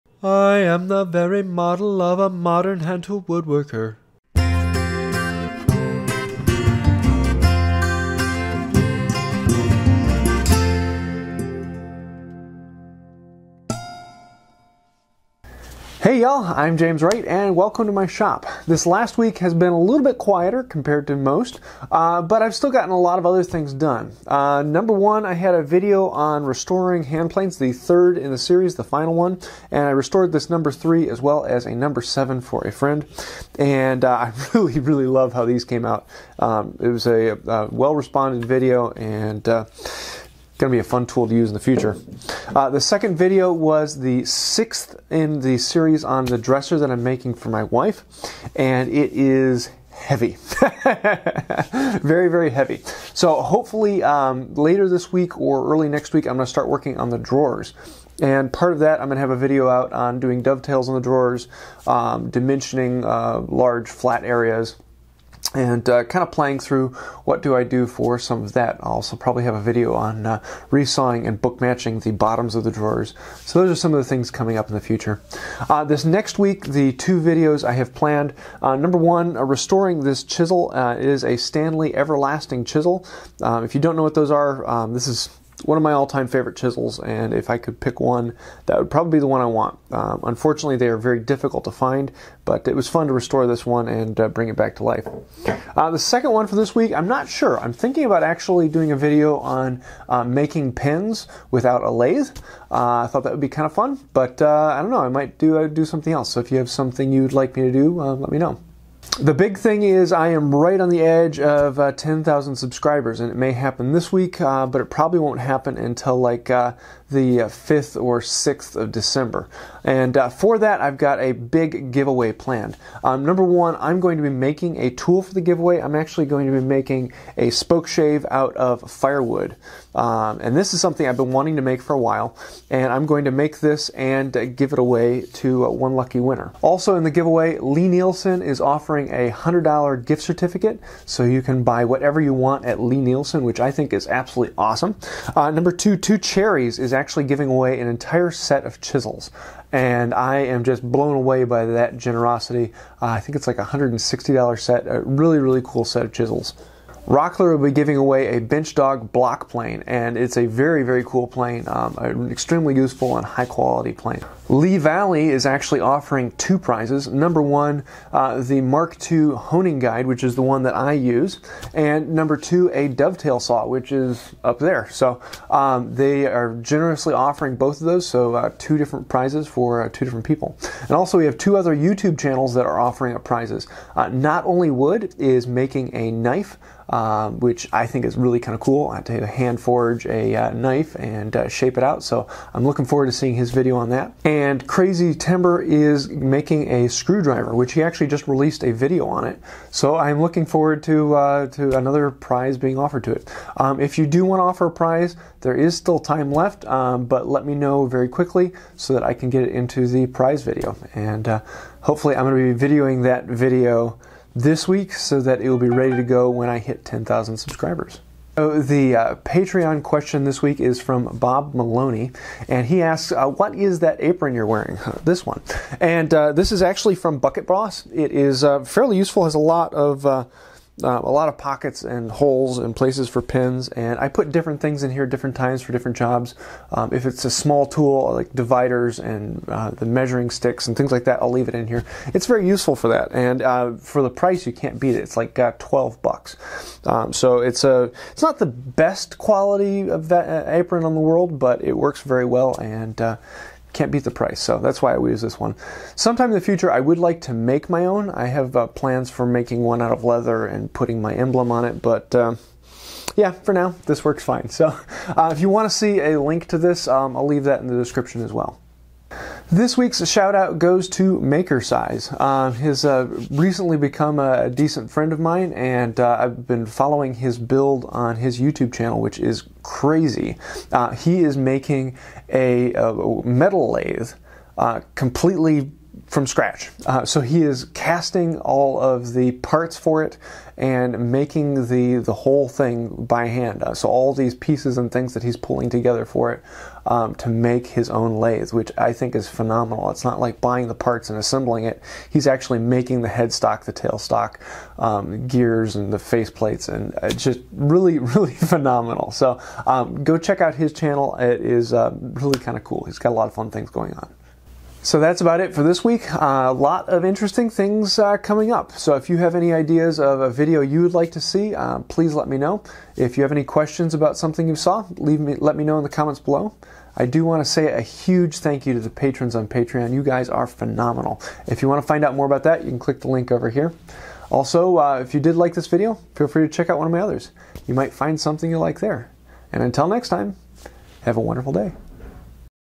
Report, 10K Giveaway, Hand Plane Restore, White Oak Dresser, Chisel restore, Making a spoke shave
One week in the Wood By Wright shop!

The question of the week: what apron are you wearing.
Bucket Boss

Two Cherries Chisel set

You Can find me:
Beme: WoodByWright
Periscope: @WoodByWright